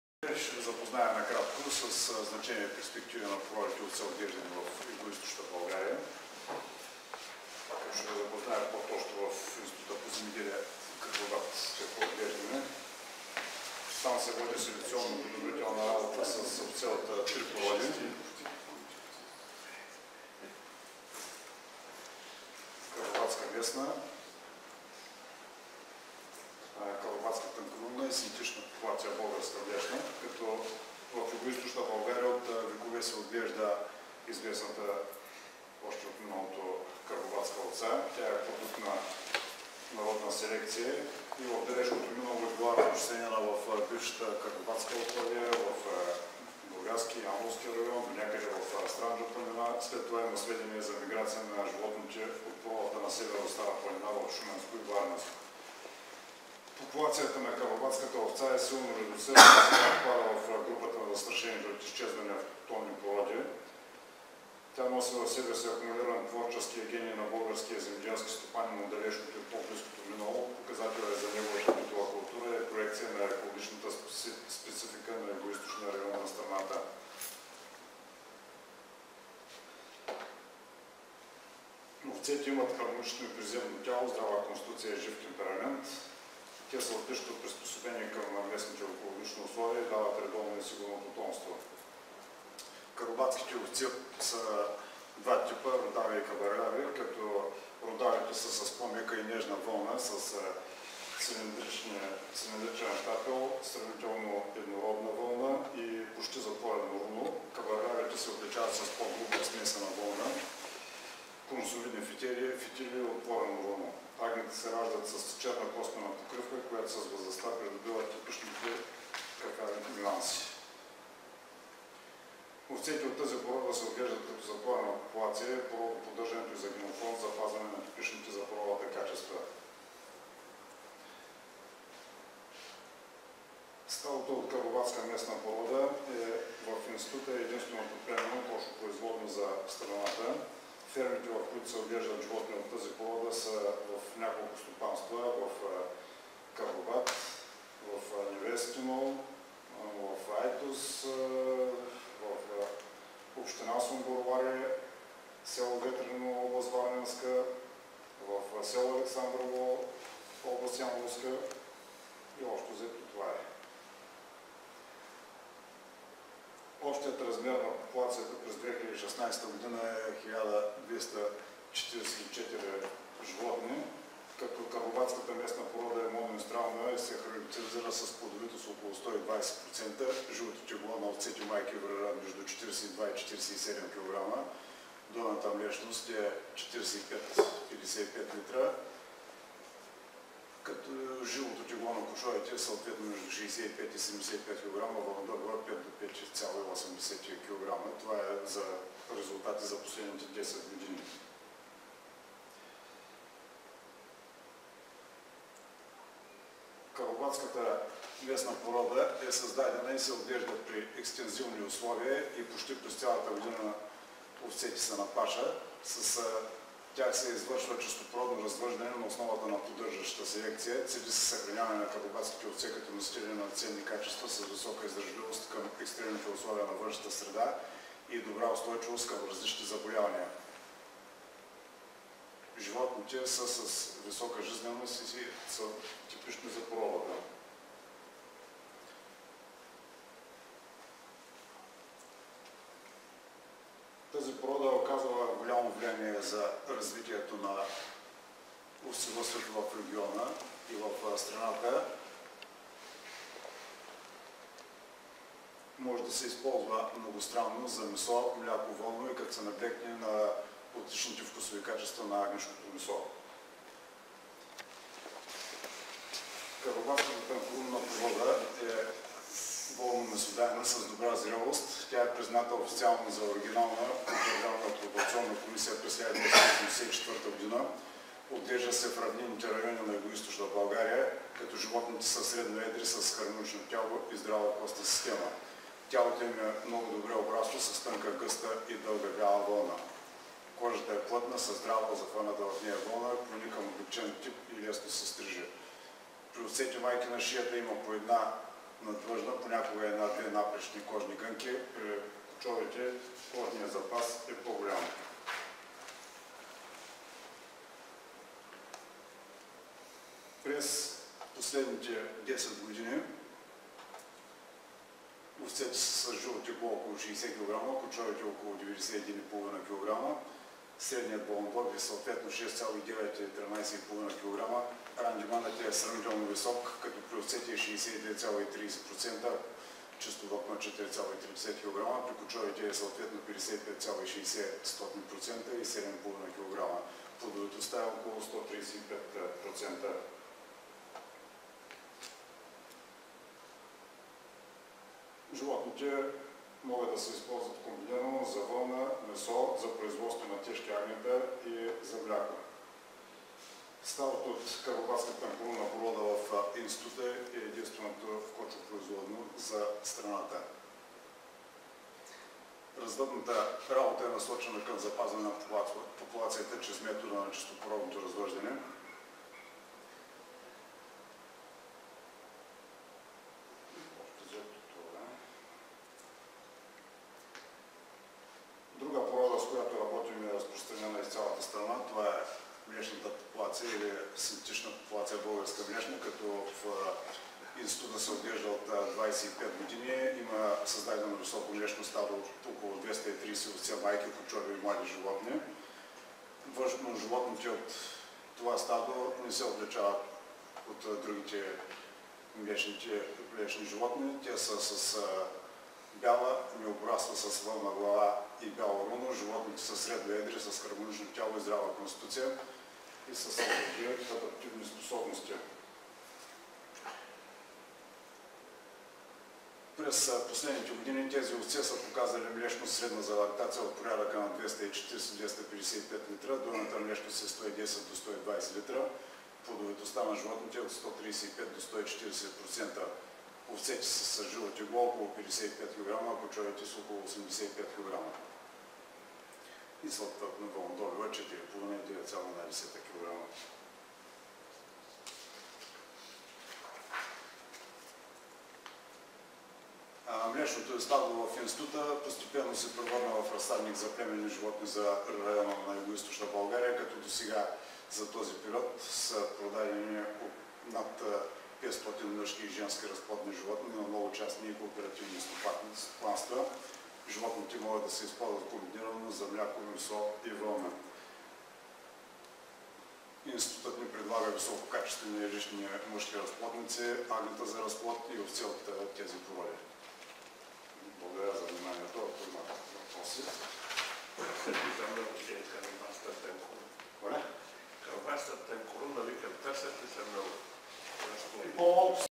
Значение и перспективи на породи овце, отглеждани в Югоизточна България-Агра 2017 г.
Значение и перспективи на породи овце, отглеждани в Югоизточна България

Доц д-р Маргарит Илиев, ИЗ Карнобат